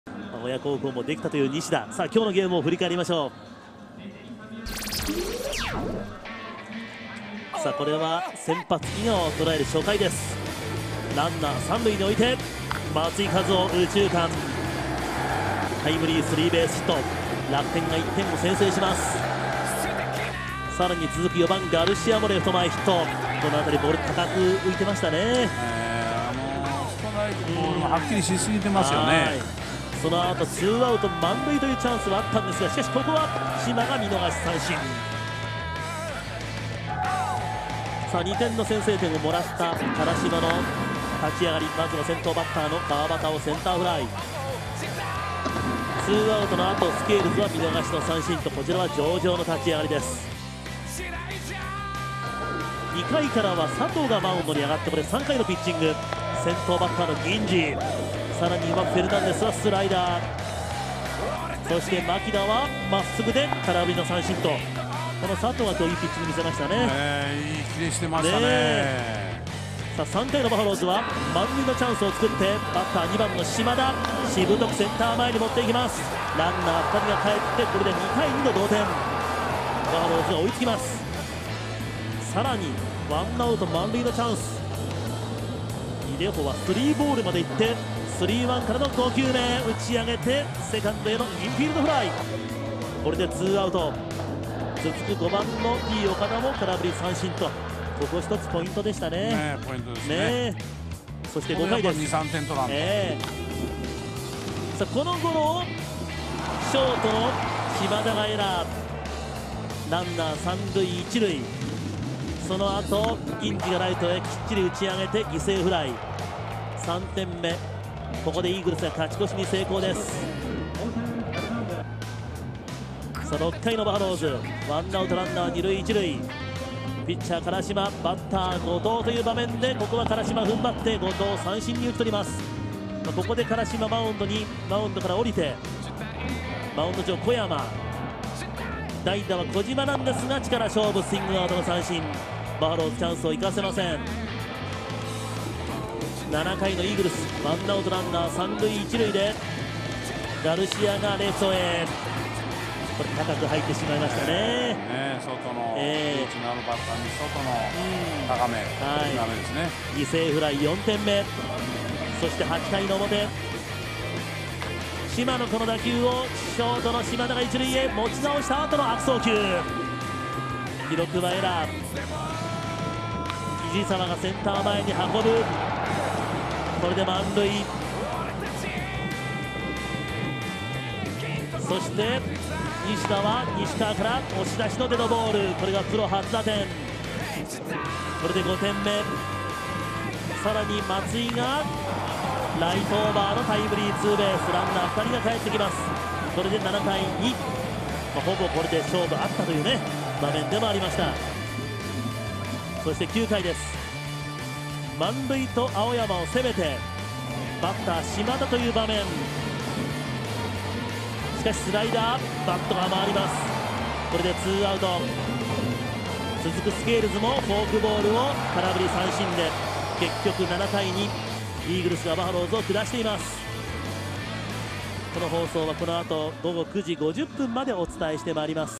2012.09.14 Bs2-7E 二桁安打で楽天が快勝！
楽天が初回にオリックスの先発・井川を打ち崩して2点を獲得。オリックスも3回に縞田のタイムリーで同点とするが、楽天は5回、7回と得点を重ね、8回には松井のタイムリーで点差を5点に広げて逃げ切った。 （9月14日 オリックス-楽天 試合ハイライト）